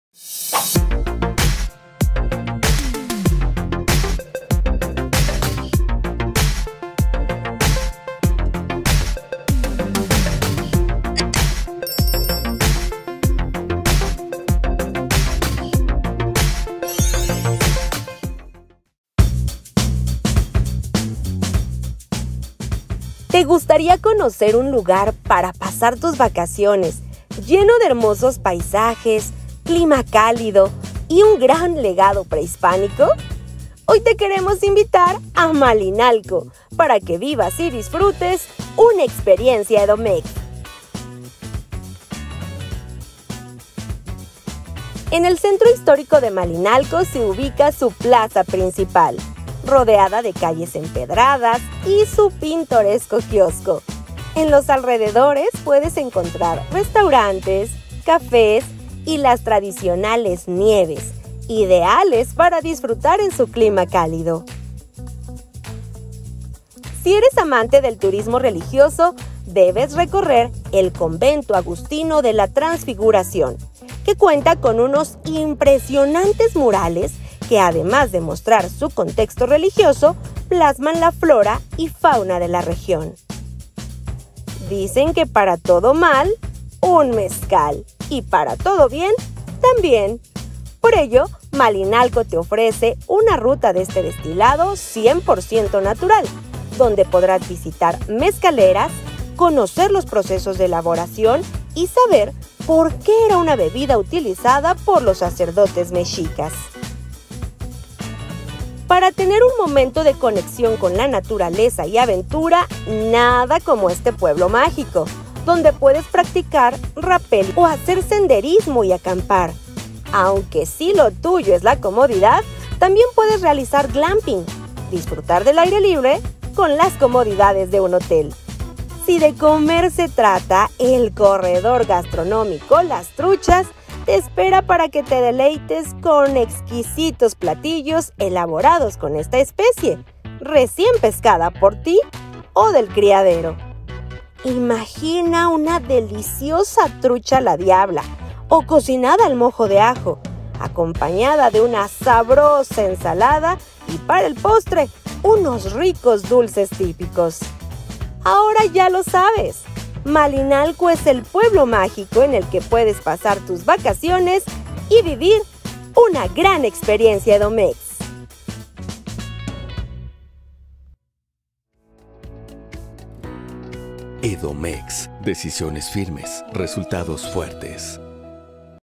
TurismoEnUnClick Malinalco
En CulturaEnUnClick tenemos una excelente propuesta para las vacaciones Malinalco, con gran cantidad de atractivos para que pases unos días increíbles, de descanso o aventura. Vive una Experiencia Edoméx.